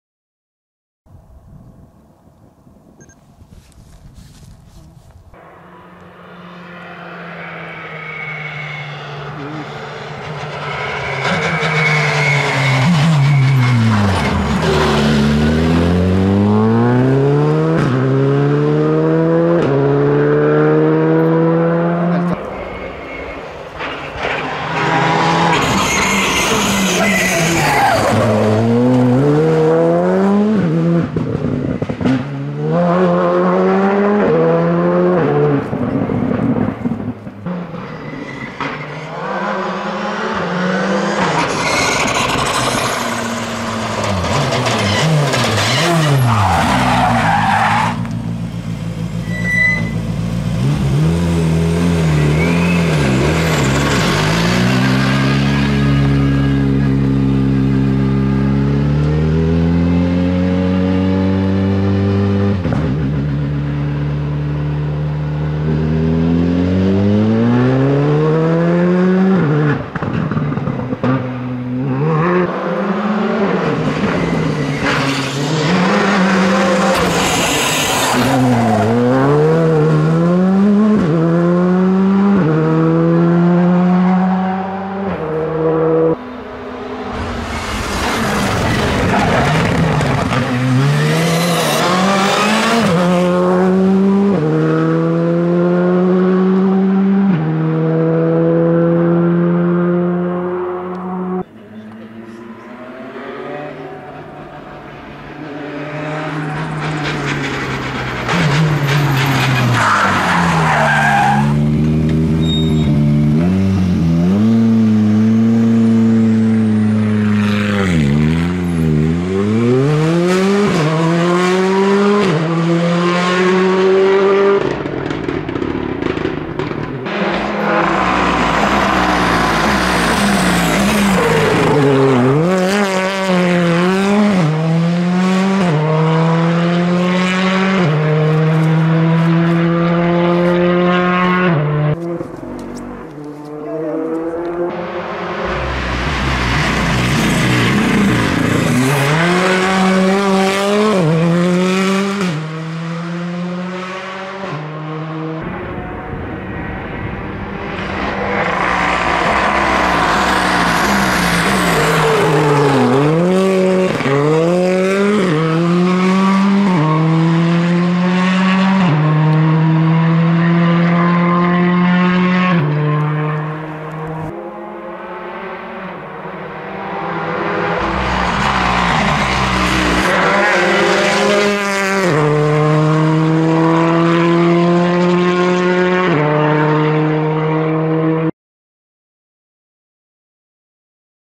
Highlights tests Pré rallye Monte Carlo 2018 Team Sarrazin by Ouhla lui
Voici quelques images de la séance d'essais du team Sarrazin avant le rallye de Monte Carlo 2018. Quentin Giordano / Vincent Landais Skoda Fabia R5 - Stéphane Sarrazin / Jacques-Julien Renucci Hyundai I20 R5